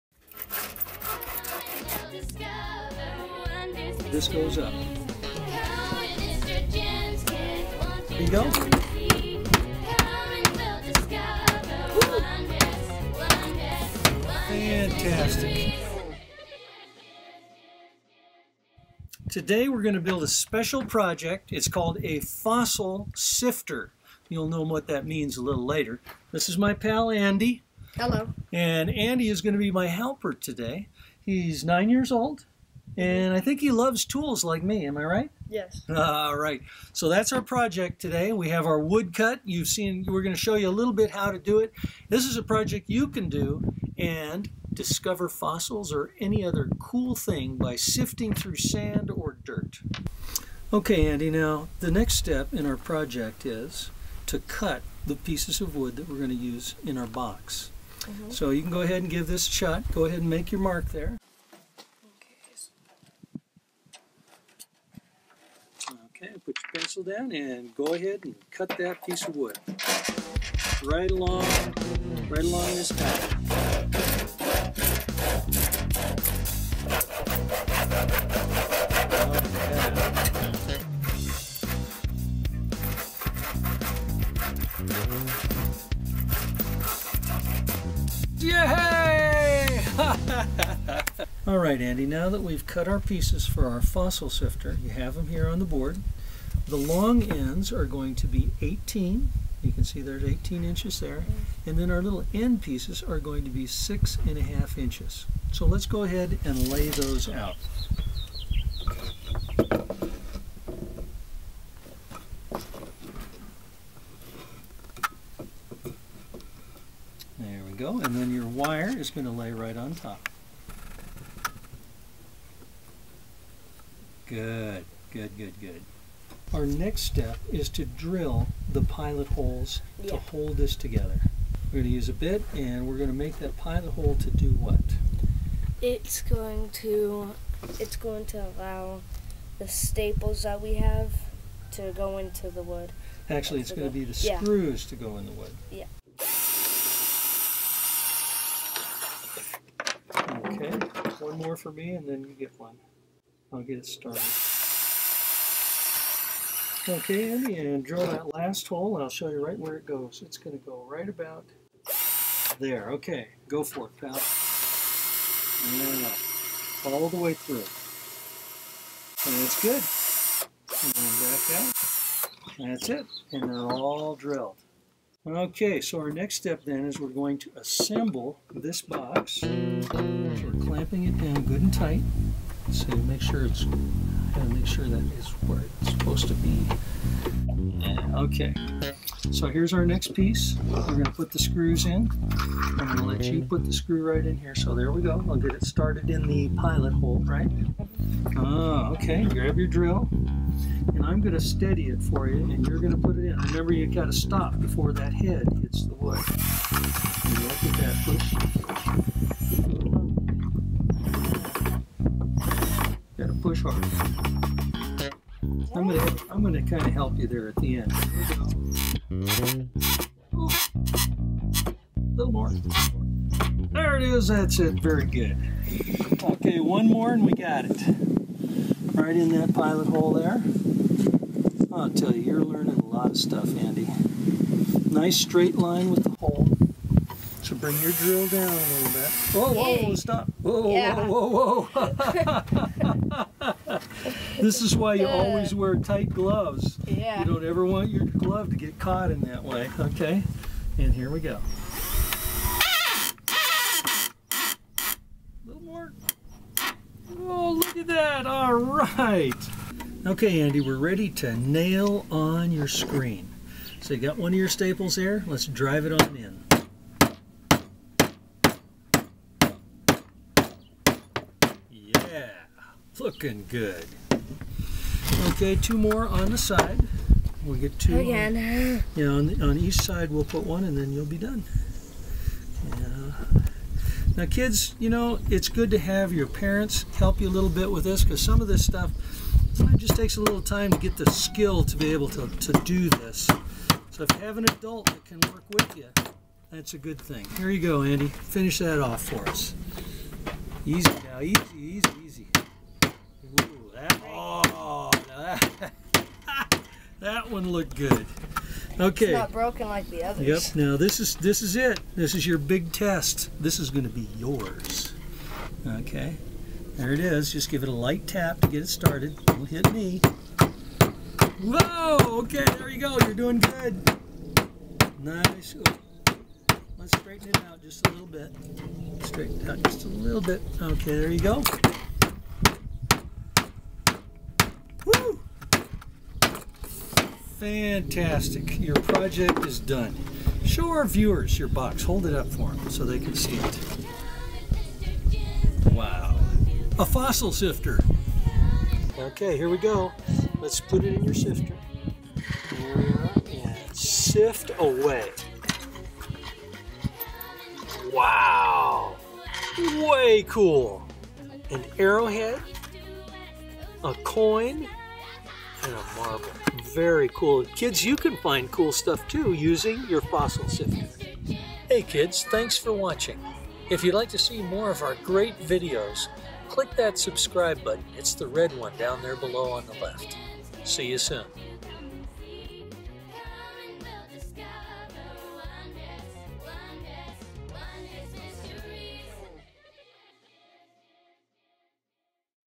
Build a Kids Fossil Sifter! | Mr. Jim's Kiddzz
Mr. Jim and Andy build a Fossil Sifter. You can too!